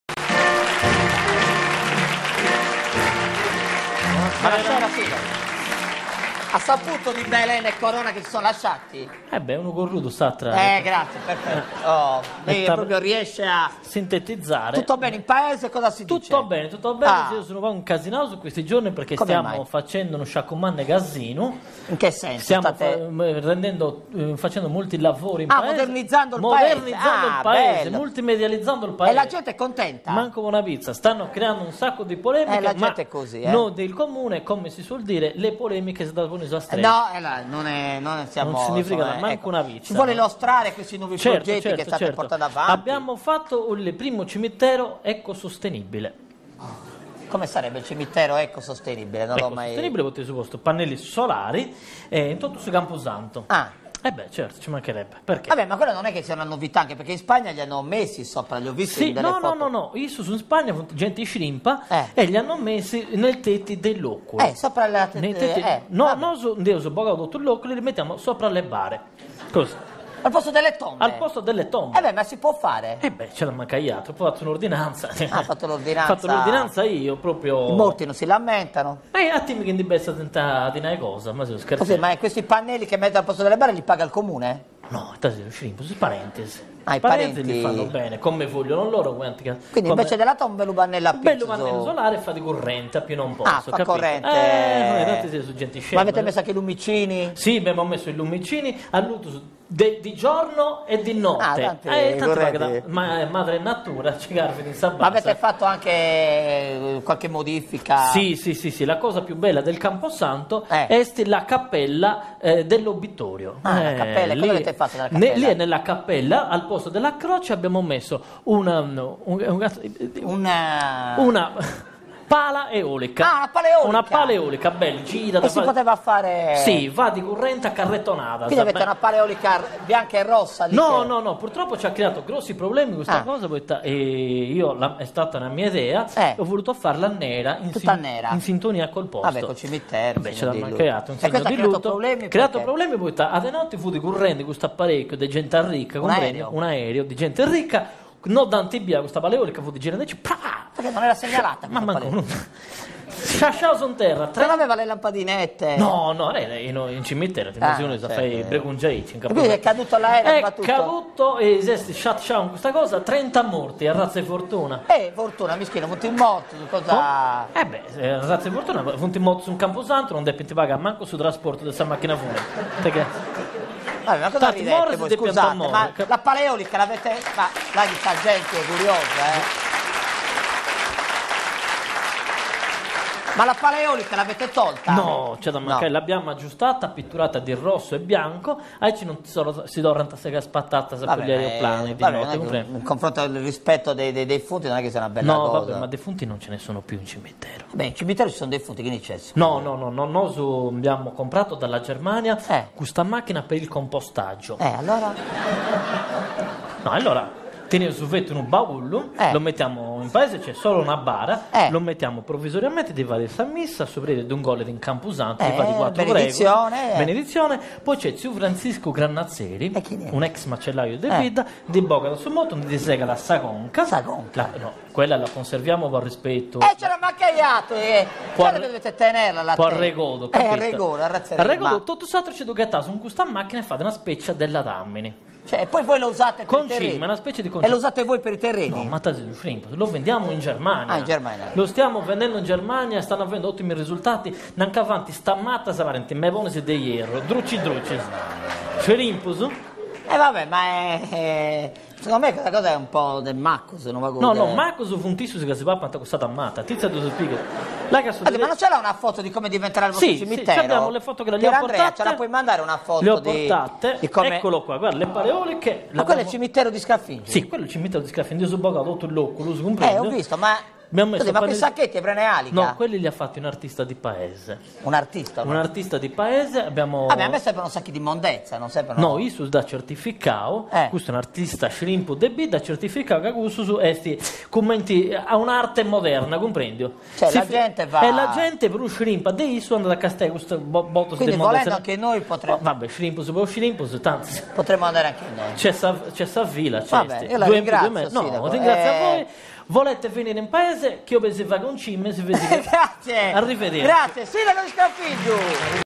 Sindaco di Scraffingiu - Cimitero ecosostenibile di Scraffingiu
Alessandro Pili interpreta il sindaco di Scraffingiu.nella puntata del 23 aprile 2012